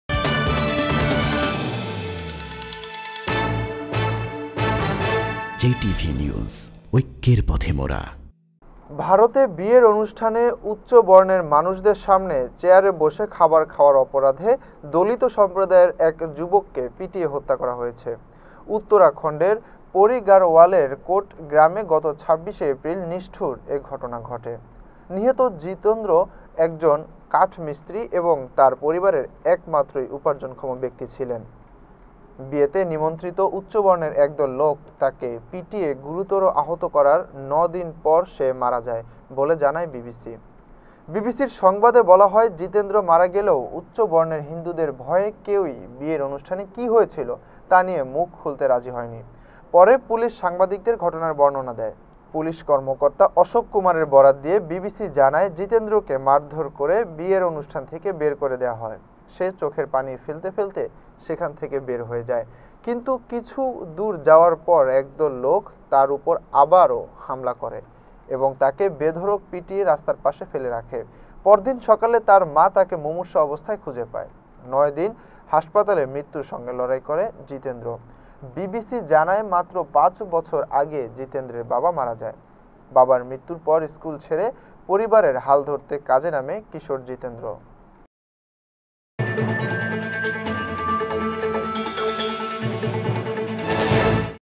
JTV | চেয়ারে বসার ‘অপরাধে’ দলিত যুবককে পিটিয়ে হত্যা | JTV News
ভারতে বিয়ের অনুষ্ঠানে উচ্চবর্ণের মানুষদের সামনে চেয়ারে বসে খাবার খাওয়ার ‘অপরাধে’ দলিত সম্প্রদায়ের এক যুবককে পিটিয়ে হত্যা করা হয়েছে। উত্তরাখণ্ডের পড়ি গাড়ওয়ালের কোট গ্রামে গত ২৬ এপ্রিল নিষ্ঠুর এ ঘটনা ঘটে। নিহত জিতেন্দ্র একজন কাঠমিস্ত্রি এবং তার পরিবারের একমাত্র উপর্জনক্ষম ব্যক্তি ছিলেন। বিয়েতে নিমন্ত্রিত উচ্চবর্ণের একদল লোক তাকে পিটিয়ে গুরুতর আহত করার নয়দিন পর সে মারা যায় বলে জানায় বিবিসি।
বিবিসির সংবাদে বলা হয়, জিতেন্দ্র মারা গেলেও উচ্চবর্ণের হিন্দুদের ভয়ে কেউ বিয়ের অনুষ্ঠানে কী হয়েছিল তা নিয়ে মুখ খুলতে রাজি হয়নি। পরে পুলিশ সাংবাদিকদের ঘটনার বর্ণনা দেয়। পুলিশ কর্মকর্তা অশোক কুমারের বরাত দিয়ে বিবিসি জানায়, জিতেন্দ্রকে মারধর করে বিয়ের অনুষ্ঠান থেকে বের করে দেওয়া হয়। সে চোখের পানি ফেলতে ফেলতে সেখান থেকে বের হয়ে যায়। কিন্তু কিছু দূর যাওয়ার পর একদল লোক তার উপর আবার হামলা করে এবং তাকে বেধড়ক পিটিয়ে রাস্তার পাশে ফেলে রাখে। পরদিন সকালে তার মা তাকে মূমুর্ষ অবস্থায় খুঁজে পায়। হাসপাতালে নয় দিন হাসপাতালে মৃত্যুর সঙ্গে লড়াই করেন জিতেন্দ্র। বিবিসি জানায়, মাত্র পাঁচ বছর আগে জিতেন্দ্রর বাবা মারা যায়। বাবার মৃত্যুর পর স্কুল ছেড়ে পরিবারের হাল ধরতে কাজে নামে কিশোর জিতেন্দ্র।